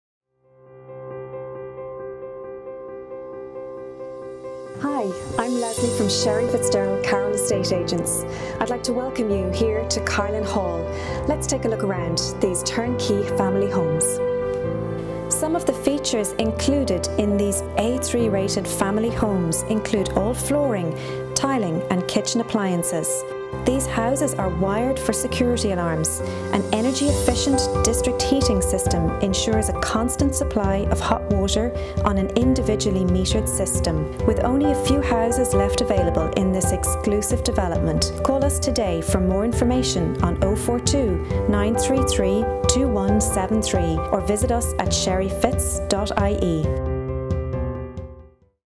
Carlinn Hall 3 Bedroom Show House
** Final Phase, last few remaining** Carlinn Hall is an exciting development of the future, offering Turn Key energy efficient homes coupled with state of the art technology inspired by the changing needs of the modern homeowner. This exclusive development boasts a delightful mix of traditional detached, semi detached and terraced homes, accommodating couples and families at all stages in their lives. Designed with a difference,

Carlinn Hall is a unique housing development of 'A3' rated Energy Efficient homes and incorporate a state of the art Bio Mass District Heating System ensuring a constant flow of piping hot water while keeping annual fuel costs to a minimum. The central heating system is linked to each homes with a separate meter allowing the home owner to customize their heating use to suit their own home needs. All homes are complete with an intelligent cabling System ensuring home owners are future proofed. Carlinn Hall offers many extra features as standard allowing the home owner enjoy added features such as the central music system, provision for broadband in all main rooms, TV cabling in each rooms and wiring for WiFi networks and home cinema surround sound systems.